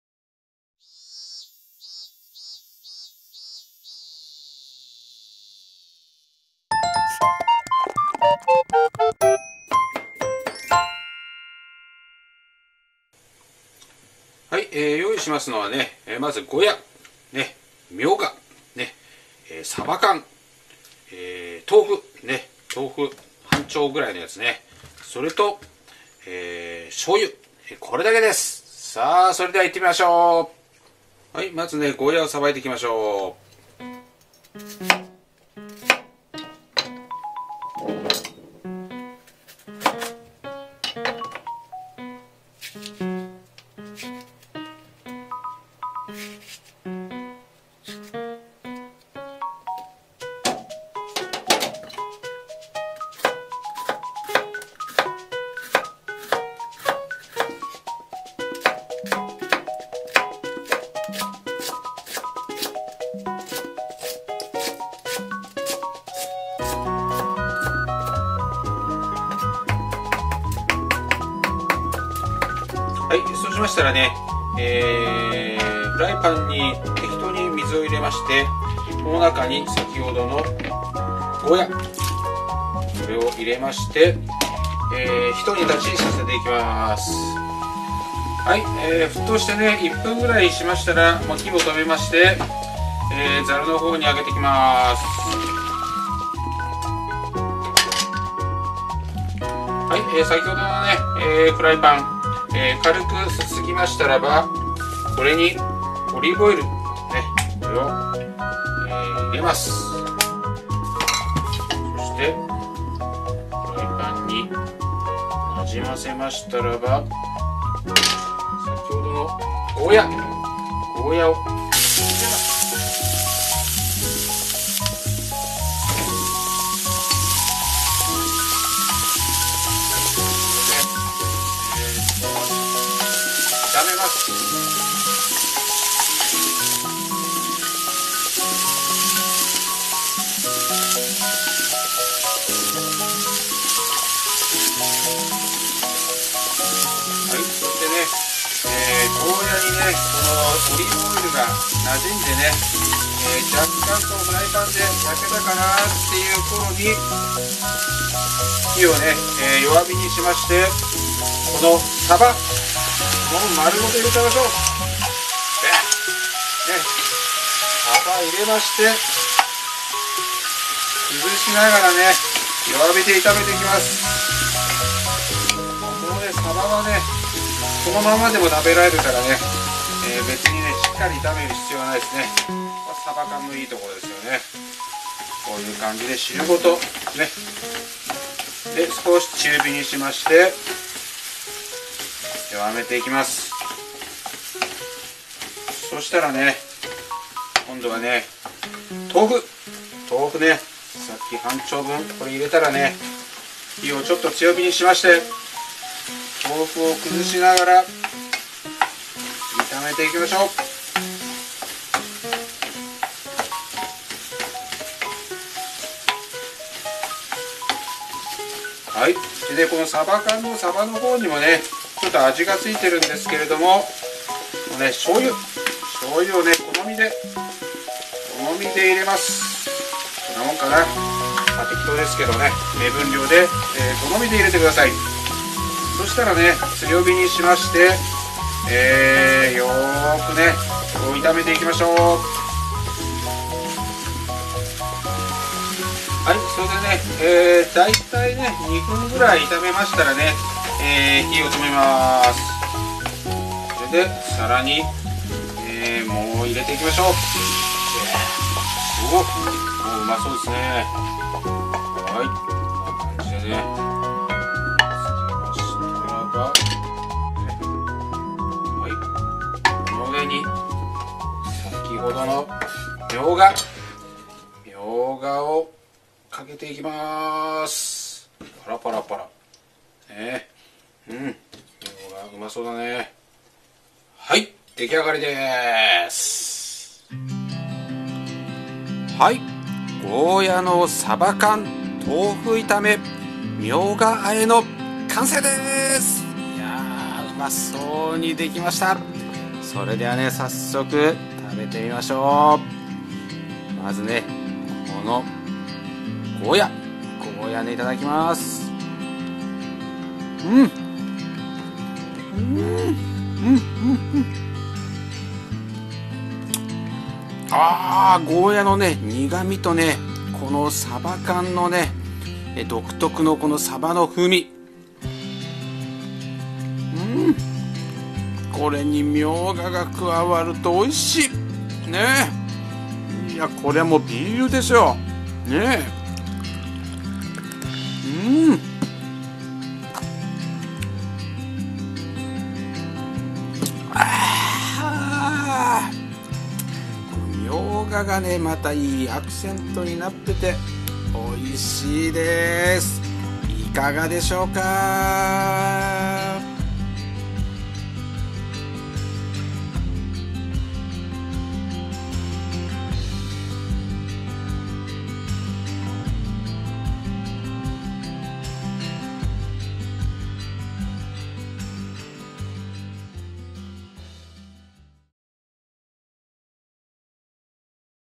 【夏料理】サバ缶とゴーヤの豆腐炒めミョウガ和え【作り方・レシピ】酒好き釣り好きゴーヤ好きの魚海藻でゴーヤ料理③【健啖隊ネット隊員】y.katsu
ゴーヤが好きで好きで家庭栽培でゴーヤを沢山育て、
冷凍庫が保存ゴーヤでいっぱいです。
簡単に直ぐに、美味しく作れて 酒の肴にもピッタシの
釣り好きですので魚海藻を使ったゴーヤ料理の紹介です。

今回はサバ缶を使ったゴーヤ料理です。
サバ缶とゴーヤを豆腐をつなぎに整えミョウガ和えで
サッパリと仕上げました。
豚肉でのチャンプルーに飽きたら是非お試しを。

音楽提供：DOVA-SYNDROME、YouTube ML